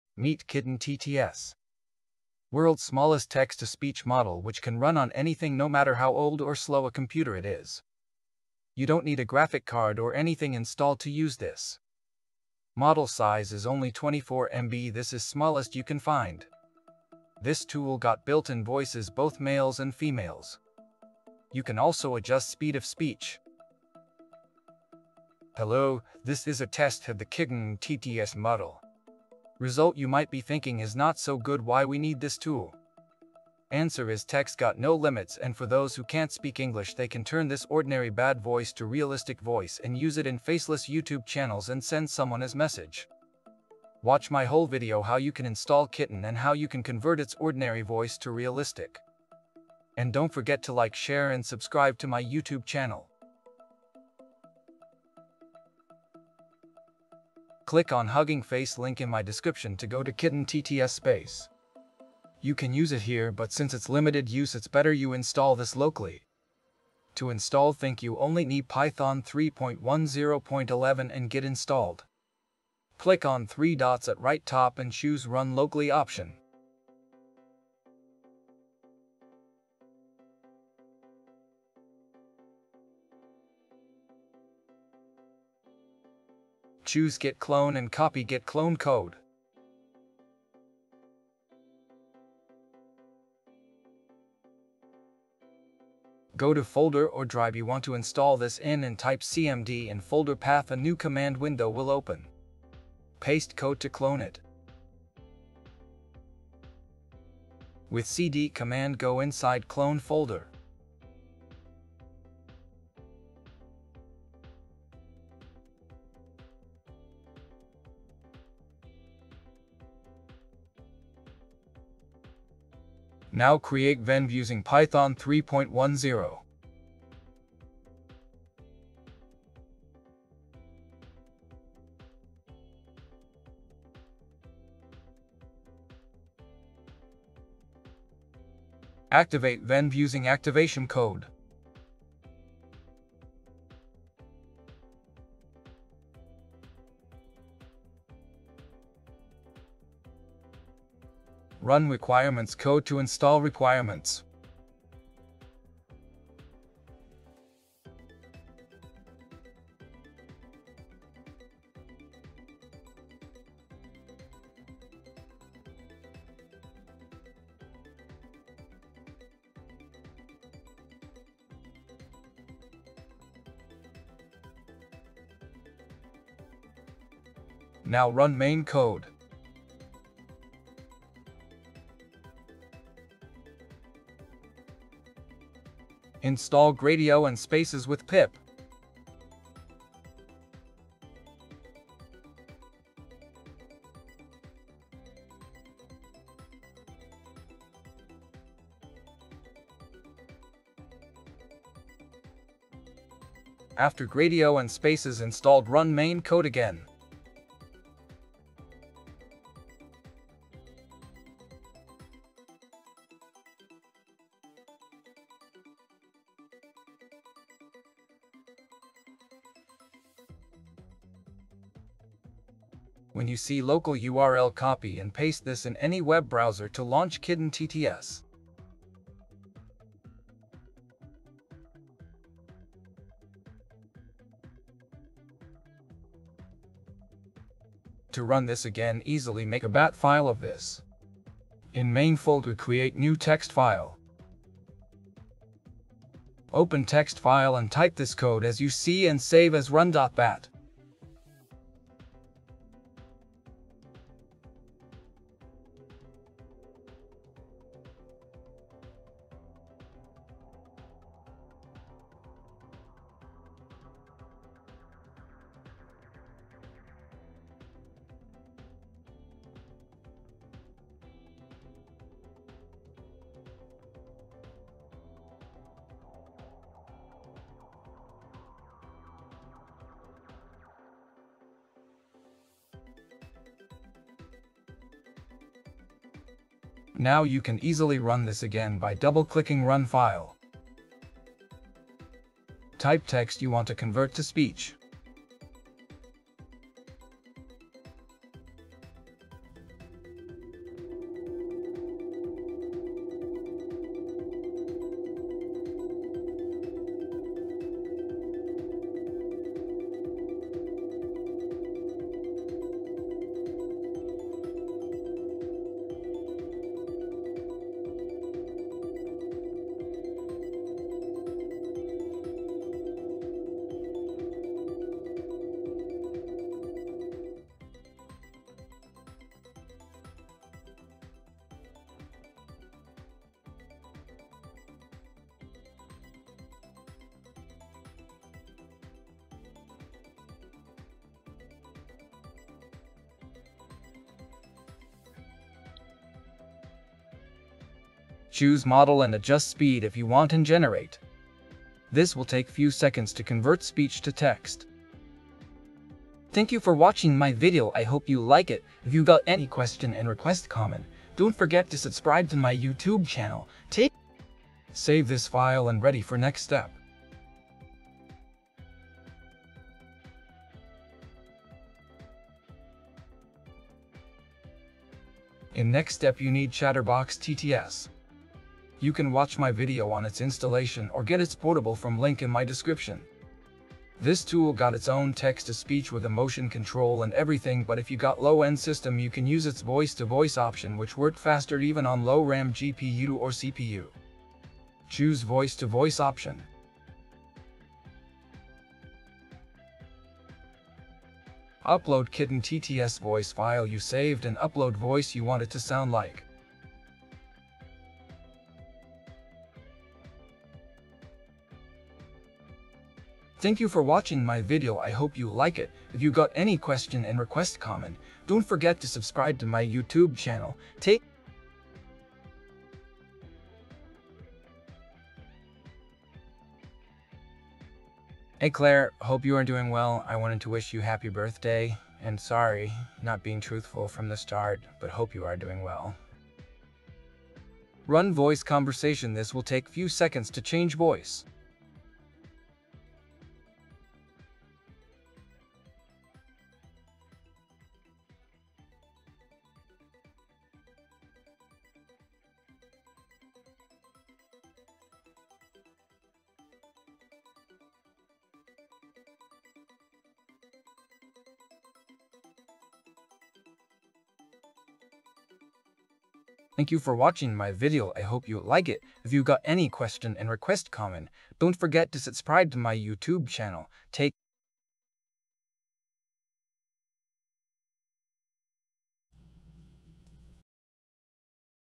KittenTTS: World’s Smallest TTS Model That Runs on Anything! 🐱💬
Turn text into real-sounding speech using the tiniest, most portable TTS model out there —  Whether you're on a low-end device, Raspberry Pi, or building a lightweight AI assistant, this TTS model just works with amazing voice quality.

✅ No GPU Needed
✅ Ultra Small Size
✅ Realistic Voice Output
✅ Runs Offline o

🎧 Whether you're building voicebots, assistants, or just experimenting with AI speech—this tool is for you.